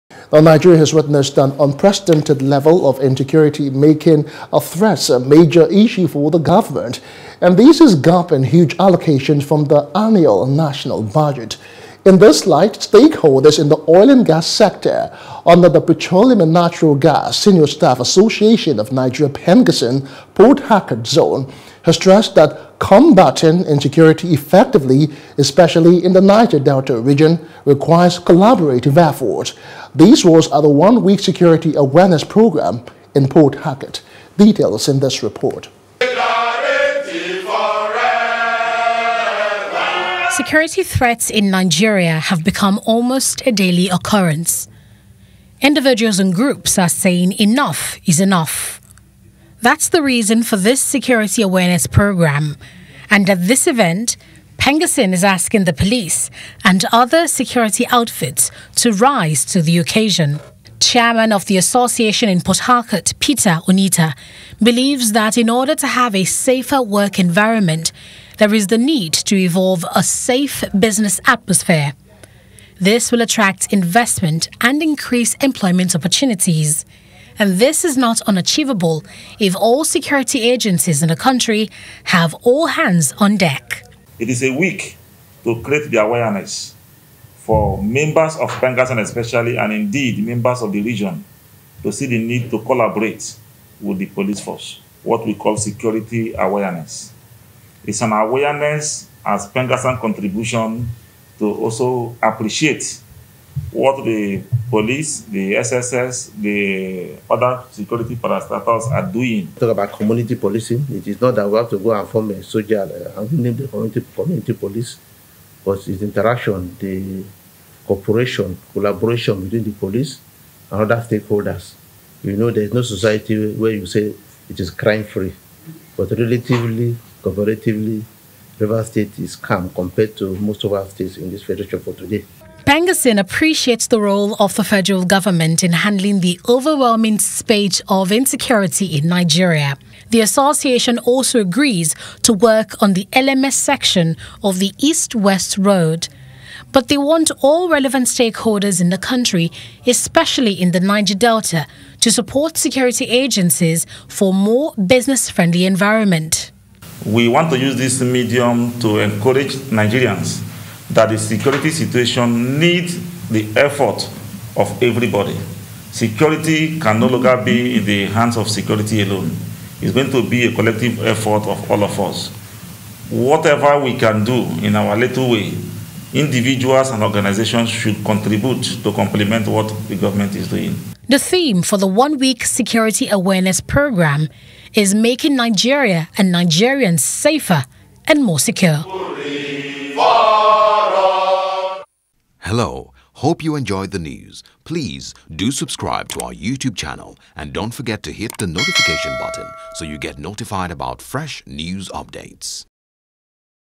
Collaborative Efforts Advised For Safer Economic Environment | NEWS
Nigeria has witnessed an unprecedented level of insecurity, making security threats a major issue for the government.